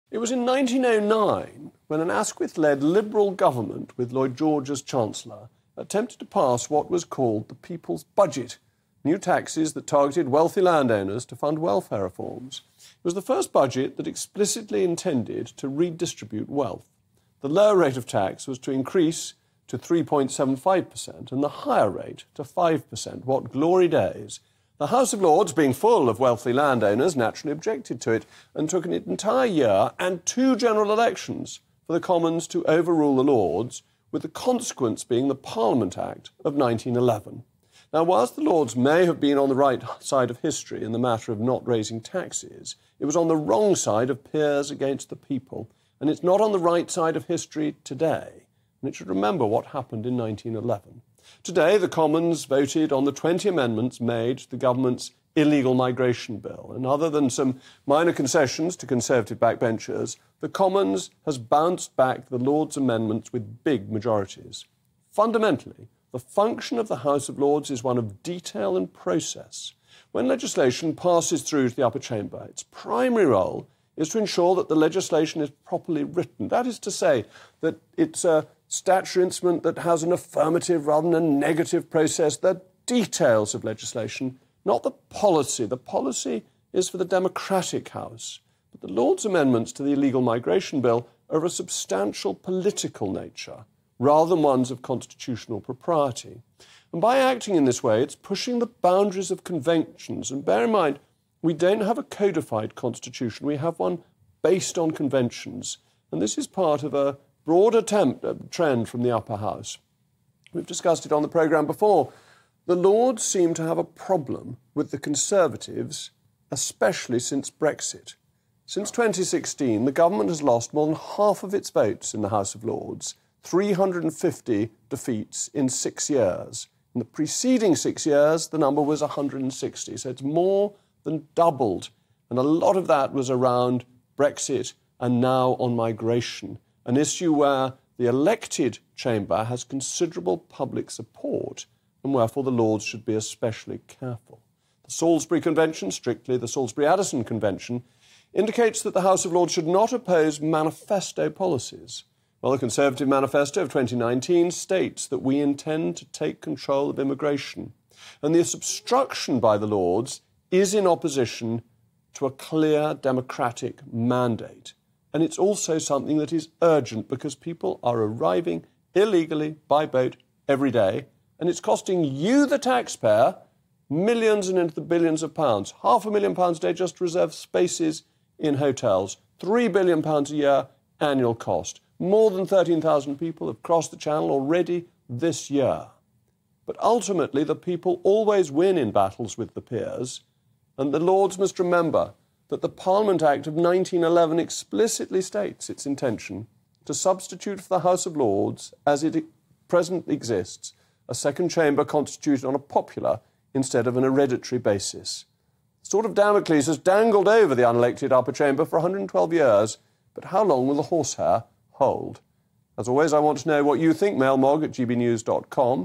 Jacob Rees-Mogg: The House of Lords seem to have a problem with the Government.
‘The Lords seem to have a problem with the Conservatives, especially since Brexit.’

Jacob Rees-Mogg says the House of Lords are ‘pushing the boundaries’ of the constitution with their opposition to the Government’s illegal migration bill.